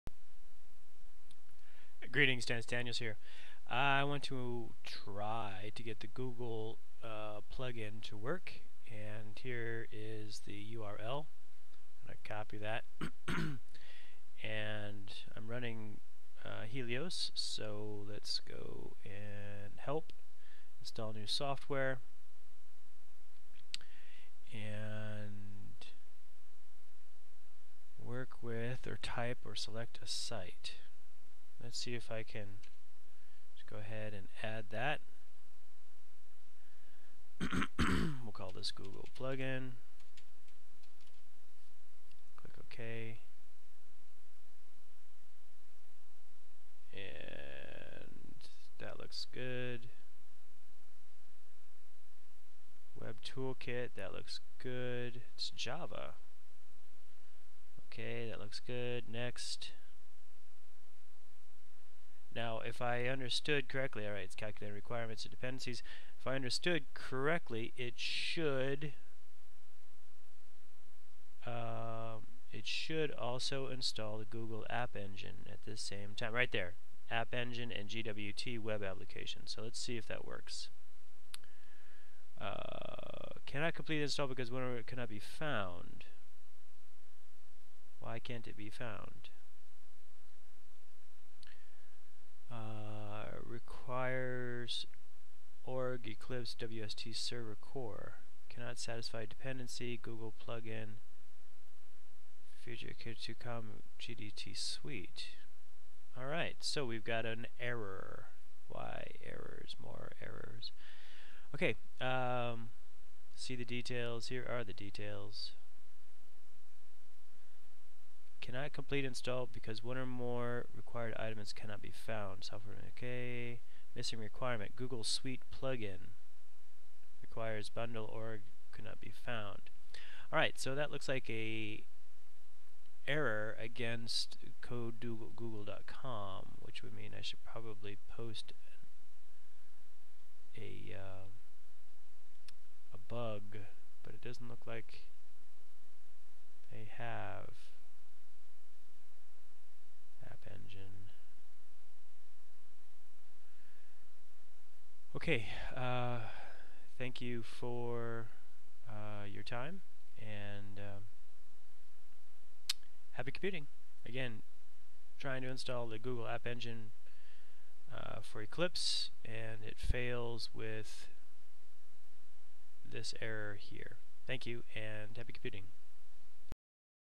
installing google plugin in Eclipse fails with error MESSAGE Cannot satisfy dependency
!ENTRY org.eclipse.ui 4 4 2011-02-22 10:25:27.302
!MESSAGE Invalid preference page path: Field Assist

!ENTRY org.eclipse.equinox.p2.operations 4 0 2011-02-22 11:00:19.187
!MESSAGE Operation details
!SUBENTRY 1 org.eclipse.equinox.p2.director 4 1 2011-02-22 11:00:19.187
!MESSAGE Cannot complete the install because one or more required items could not be found.
!SUBENTRY 2 org.eclipse.equinox.p2.director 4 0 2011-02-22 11:00:19.187
!MESSAGE Software being installed: Google Plugin for Eclipse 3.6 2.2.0.v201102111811 (com.google.gdt.eclipse.suite.e36.feature.feature.group 2.2.0.v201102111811)
!MESSAGE Missing requirement: Google Suite Plugin 2.2.0.v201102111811 (com.google.gdt.eclipse.suite 2.2.0.v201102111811) requires 'bundle org.eclipse.wst.server.core 0.0.0' but it could not be found
!SUBENTRY 2 org.eclipse.equinox.p2.director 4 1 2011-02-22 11:00:19.187
!MESSAGE Cannot satisfy dependency:
!SUBENTRY 3 org.eclipse.equinox.p2.director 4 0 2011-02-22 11:00:19.187
!MESSAGE From: Google Plugin for Eclipse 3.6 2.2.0.v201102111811 (com.google.gdt.eclipse.suite.e36.feature.feature.group 2.2.0.v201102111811)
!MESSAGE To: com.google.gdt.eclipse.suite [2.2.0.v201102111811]

!ENTRY org.eclipse.equinox.p2.operations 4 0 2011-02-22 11:00:19.196
!MESSAGE Operation details
!SUBENTRY 1 org.eclipse.equinox.p2.director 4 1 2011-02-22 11:00:19.196
!SUBENTRY 2 org.eclipse.equinox.p2.director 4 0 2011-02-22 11:00:19.196
!MESSAGE Missing requirement: Google Suite Plugin 2.2.0.v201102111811
here is the error message: